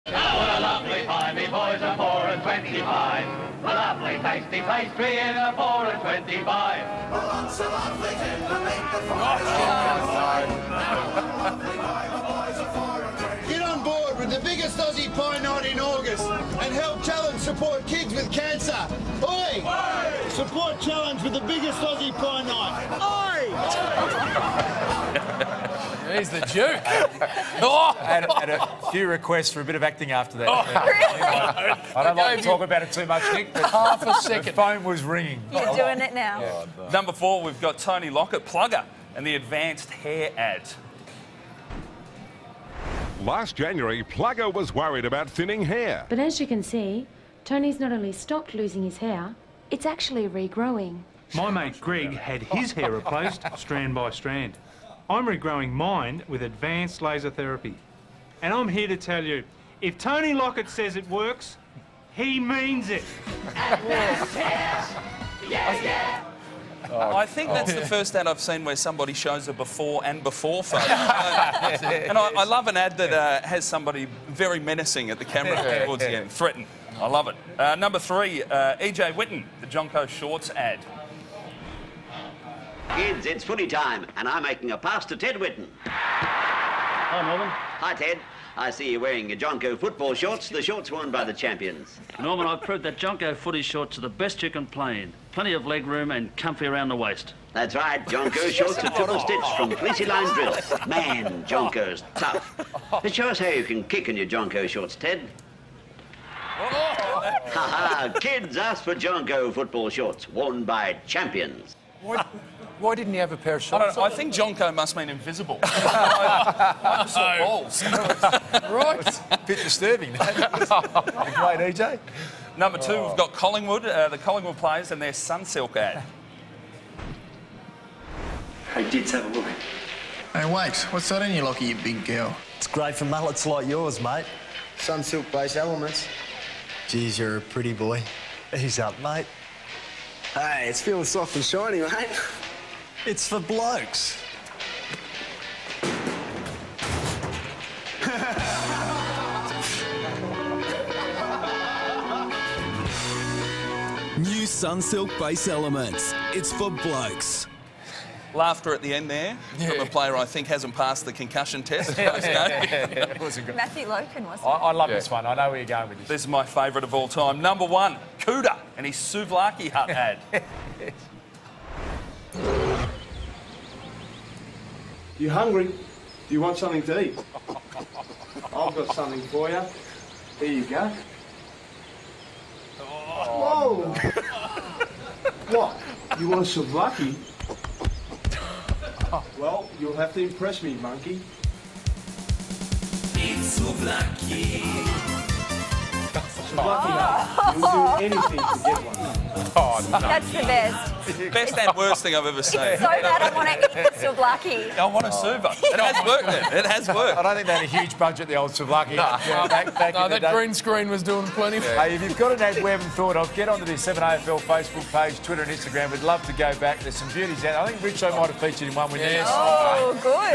The Kick | Top 5 Footy Ads of all time
What are the best AFL ads ever made? The Kick team run down the Top 5.